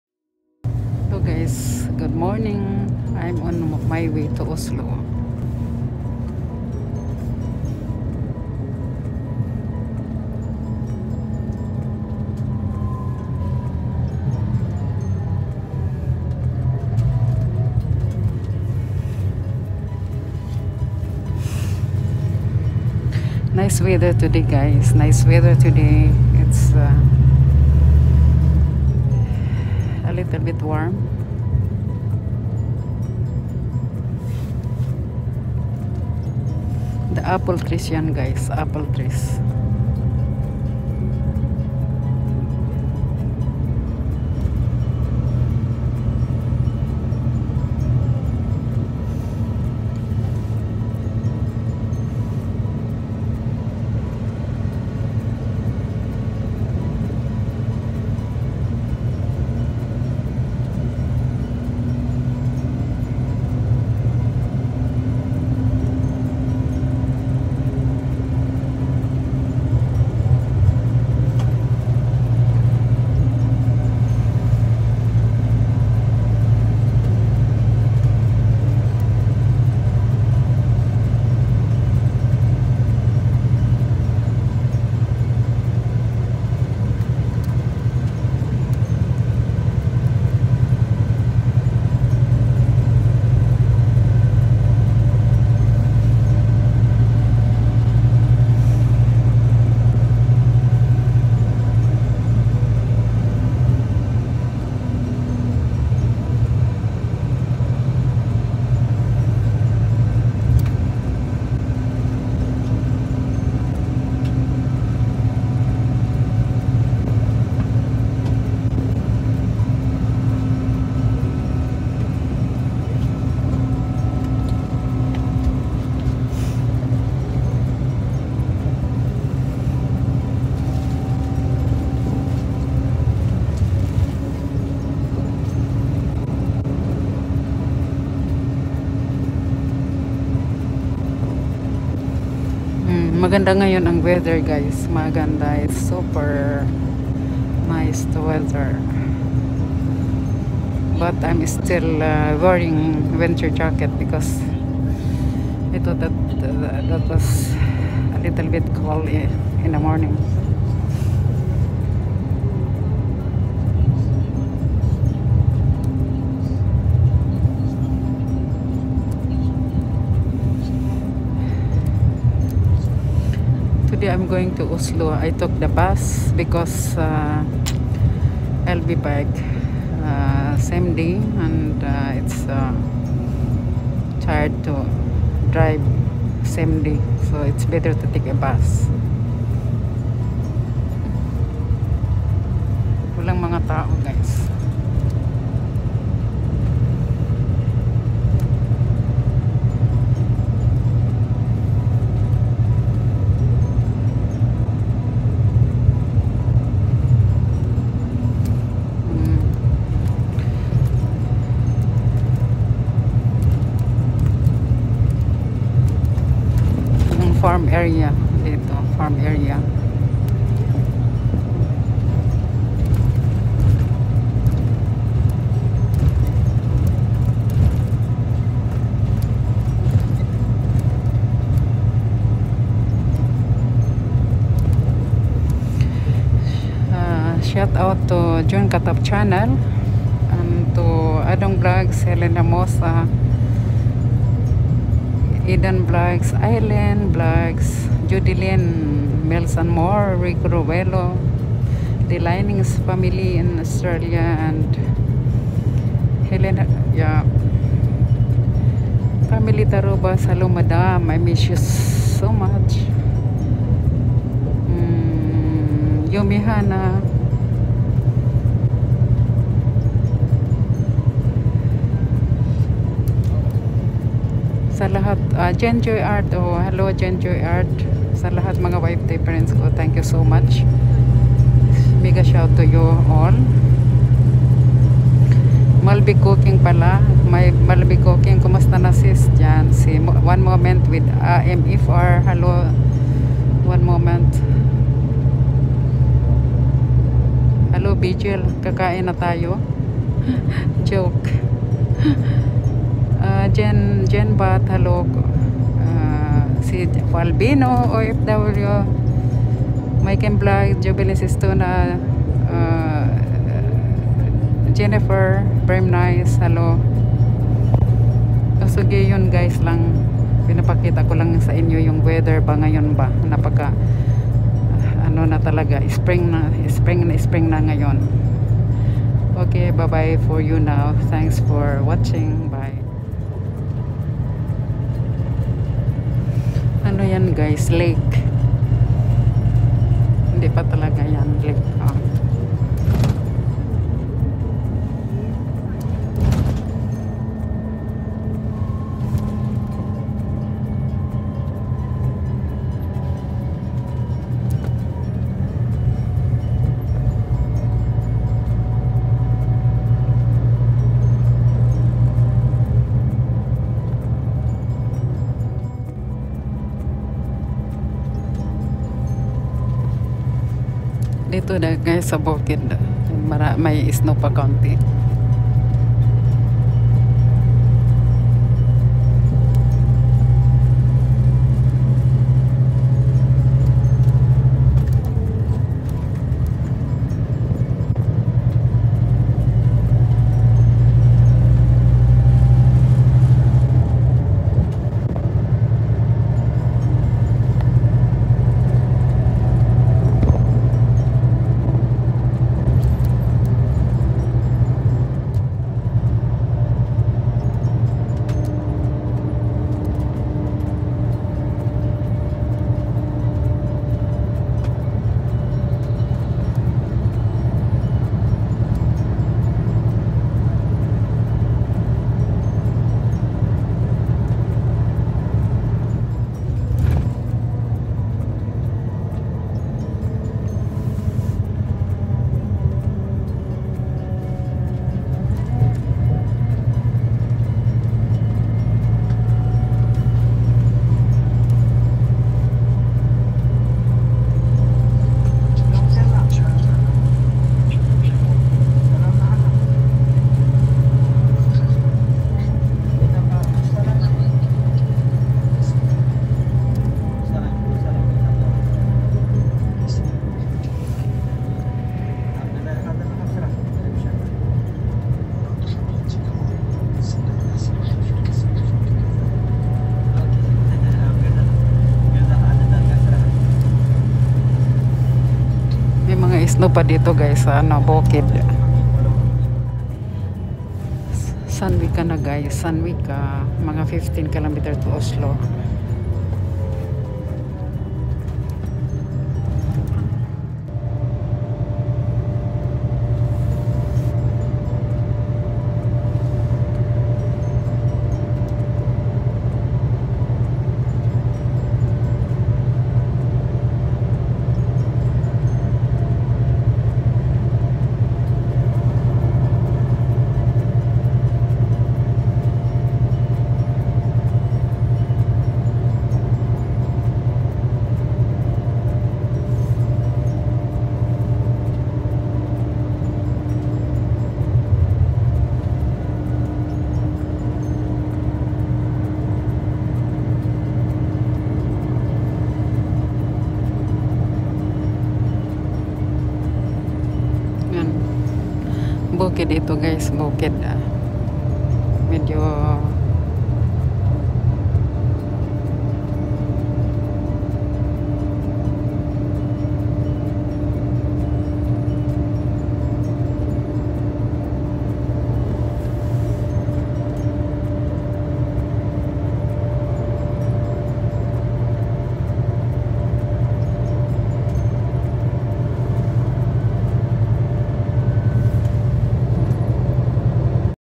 Beautiful Norway from Hønefoss to Oslo
On my way to Oslo by buss.This  time guys is spring in Norway and this end of march we change the time for ex the time here now is 12 noon and the time in the Phils is 6 pm.Just watch this video guys and u can see the beautiful nature of Norway.Just told u before that Norway is land of fjords.Obs sa mga bagohan pls panoorin ninyo ng maigi ang video tapos komento lang kung ano nakita ninyo sa video.pls don't forget to subs. guys ty.